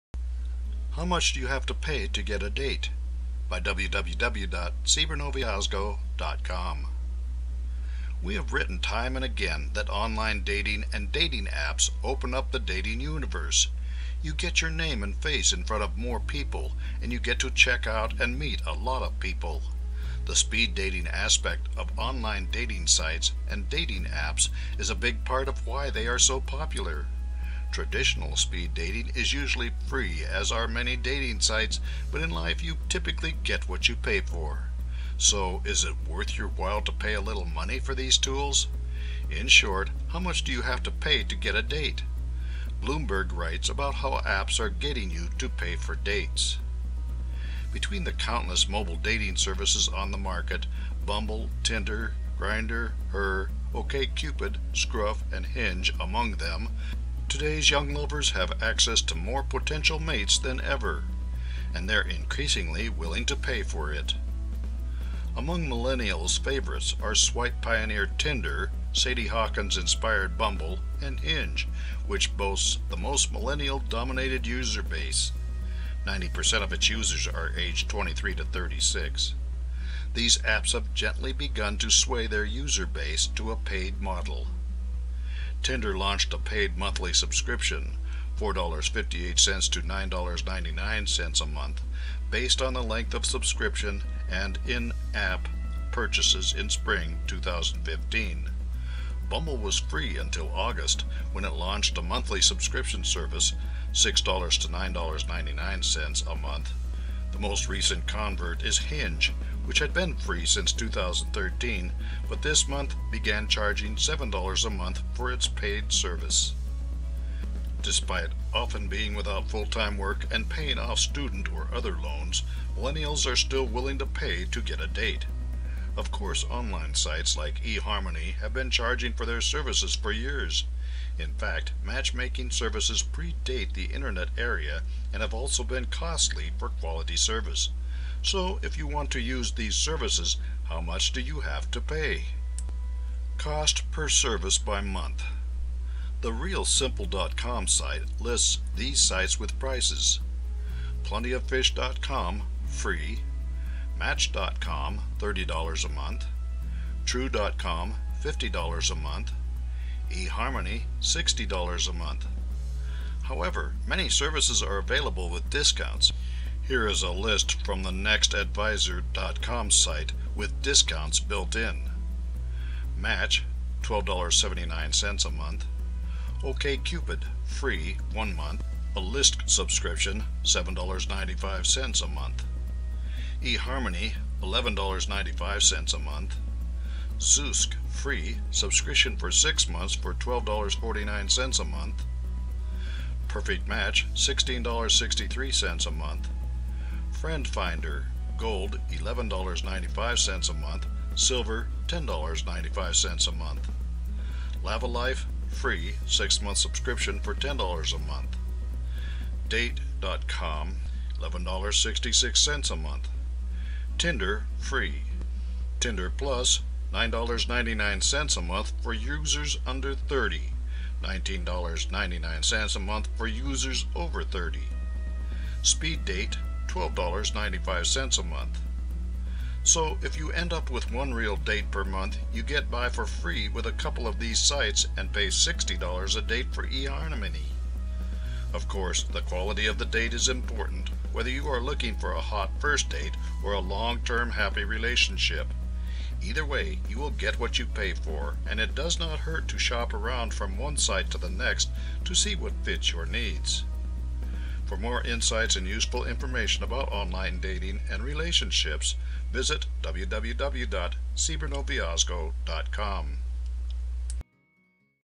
How Much Do You Have to Pay to Get a Date?
We have written time and again that online dating and dating apps open up the dating universe. You get your name and face in front of more people and you get to check out and meet a lot of people. The speed dating aspect of online dating sites and dating apps is a big part of why they are so popular. Traditional speed dating is usually free as are many dating sites but in life you typically get what you pay for. So, is it worth your while to pay a little money for these tools? In short how much do you have to pay to get a date? Bloomberg writes about how apps are getting you to pay for dates.

Between the countless mobile dating services on the market-Bumble, Tinder, Grindr, Her, OkCupid, Scruff, and Hinge among them-today's young lovers have access to more potential mates than ever. And they're increasingly willing to pay for it.

Among millennials' favorites are swipe pioneer Tinder, Sadie Hawkins-inspired Bumble, and Hinge, which boasts the most millennial-dominated user base. (Ninety percent of its users are aged 23 to 36.) These apps have gently begun to sway their user base to a paid model.

Tinder launched a paid monthly subscription-$4.58 to $9.99 a month, based on the length of subscription-and in-app purchases in spring 2015. Bumble was free until August, when it launched a monthly subscription service-$6 to $9.99 a month. The most recent convert is Hinge, which had been free since 2013 but this month began charging $7 a month for its paid service.

Despite often being without full time work and paying off student or other loans millennials are still willing to pay to get a date.  Of course online sites like eHarmony have been charging for their services for years. In fact match making services predate the internet era and have also been costly for quality service. So, if you want to use these services how much do you have to pay?

Cost per Service by Month

The realsimple.com site lists these sites with prices.

PlentyofFish.com: Free
Match.com: $30/month
True.com: $50/month
EHarmony: $60/month

However many services are available with discounts. Here is a list from the nextadvisor.com site with discounts built in.

Match: 12.79/month
OKCupid: Free, One month A list subscription-$7.95/month
eHarmony: $11.95/month
Zoosk: Free, subscription for 6 months for $12.49/month
PerfectMatch:$16.63/month
FriendFinder: Gold-$11.95/month, Silver-$10.95/month
Lavalife: Free, six month subscription for $10/month
Date.com: $11.66/month
Tinder: free, Tinder Plus-$9.99/month for users under 30, $19.99/month for users over 30
SpeedDate: $12.95/month

So if you end up with one real date per month you get by for free with a couple of these sites and pay $60 a date for eHarmony.

Of course the quality of the date is important whether you are looking for a hot first date or a long term happy relationship.  Either way you will get what you pay for and it does not hurt to shop around from one site to the next to see what fits your needs.